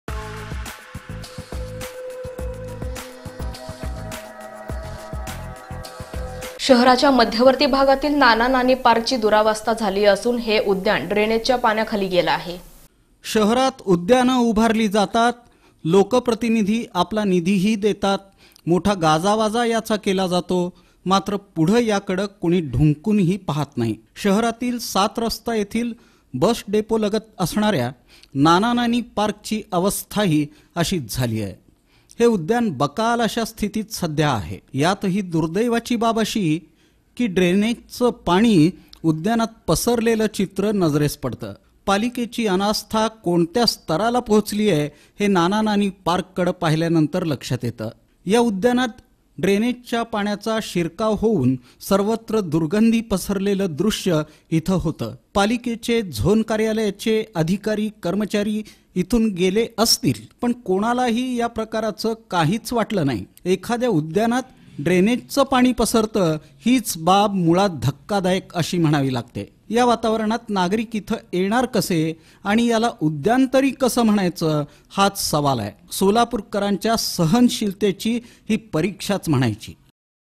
नाना नानी पार्क ड्रेनेजच्या पाण्यात... शहरातील उद्यानं बकाल... | NANA NANI PARK
नाना नानी पार्क ड्रेनेजच्या पाण्यात... शहरातील उद्यानं बकाल...